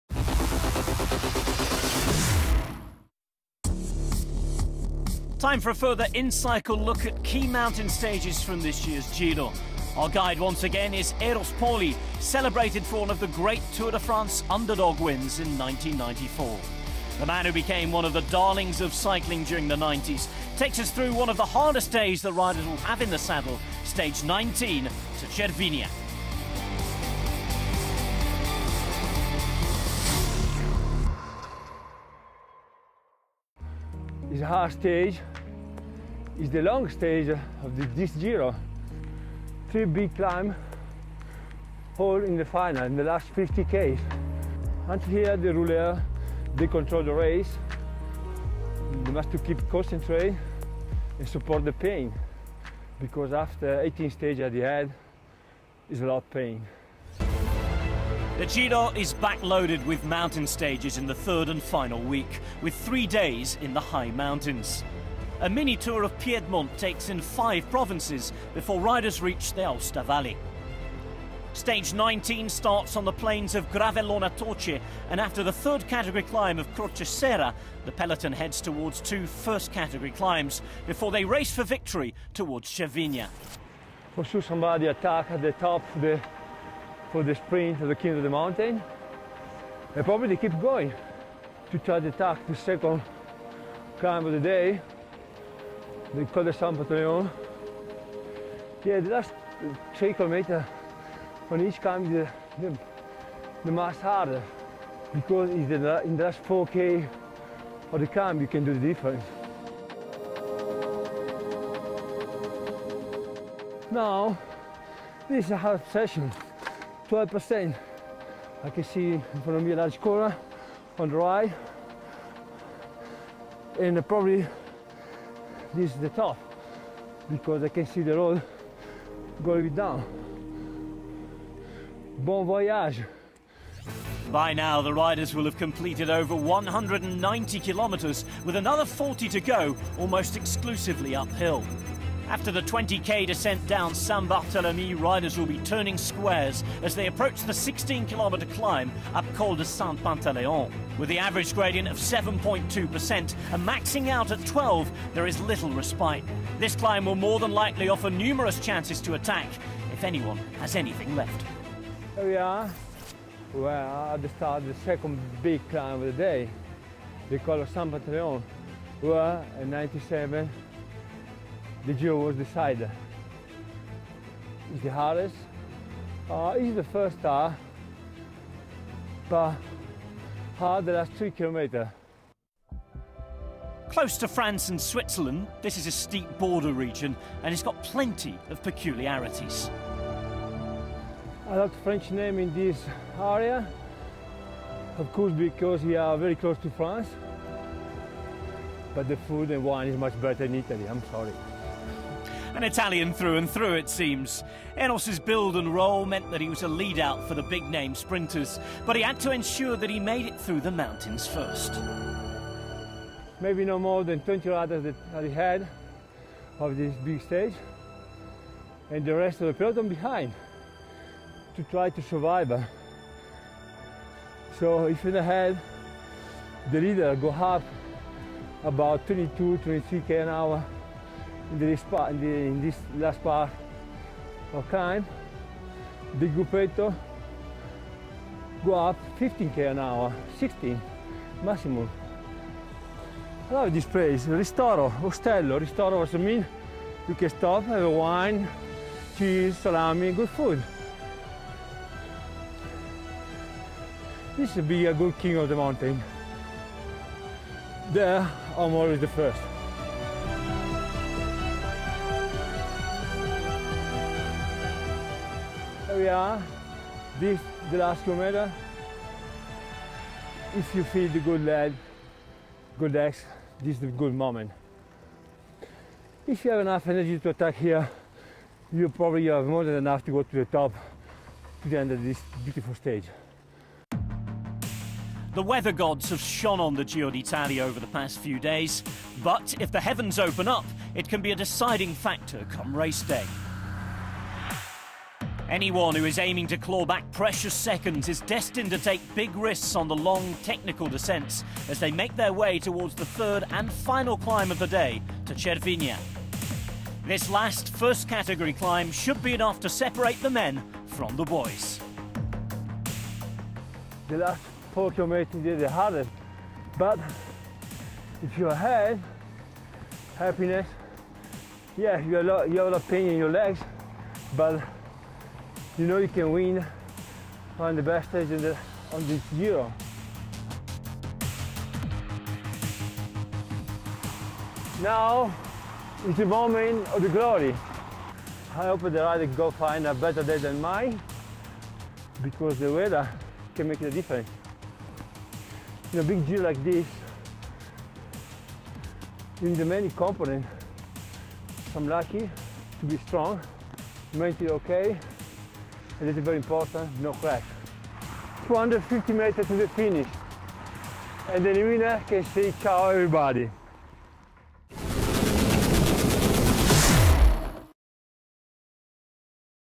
inCycle Giro d’Italia – Stage 19 Preview with Eros Poli
Tour de France stage winner Eros Poli rides through one of the upcoming, and potentially decisive mountain stages of the 2015 Giro, Stage 19